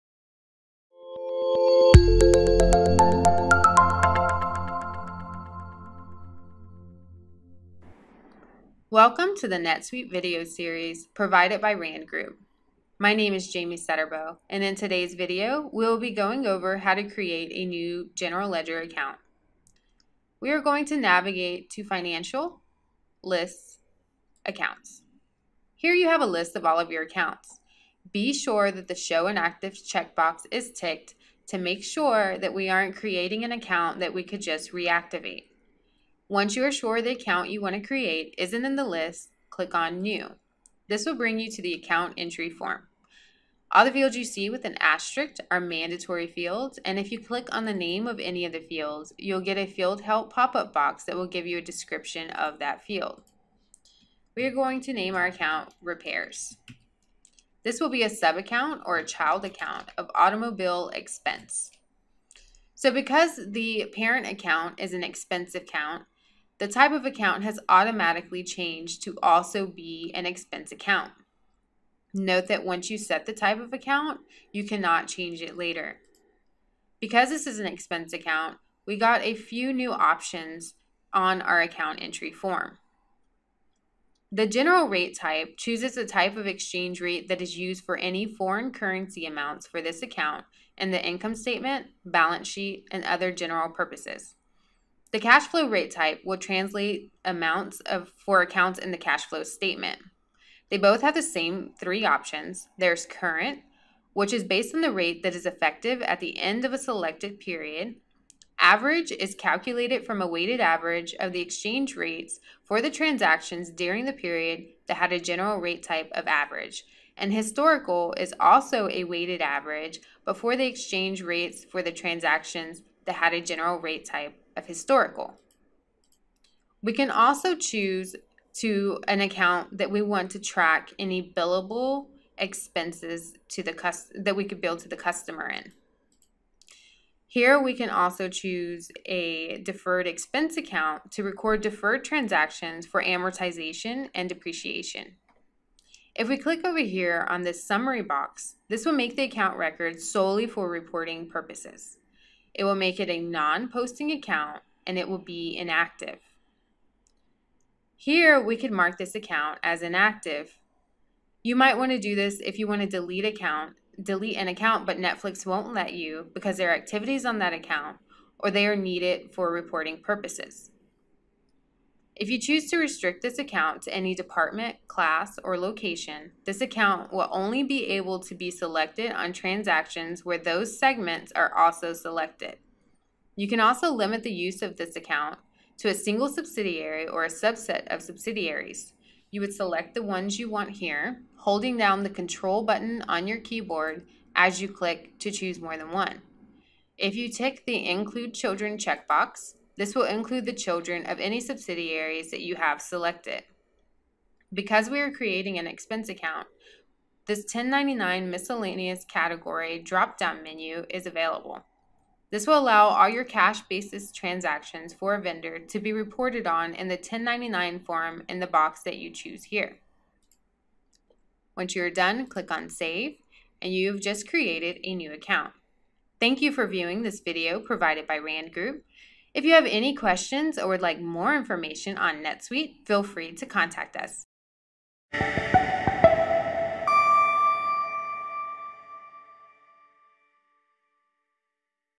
How to Create a General Ledger Account in NetSuite
The general ledger summarizes a company’s financial position. It’s the single, agreed-upon record of data for accounting teams tasked with ensuring the books balance, providing status updates on the company’s cash position, recording the information needed for financial statements and kicking off audit trails.

Learn how to create a general ledger account in NetSuite ERP in this video.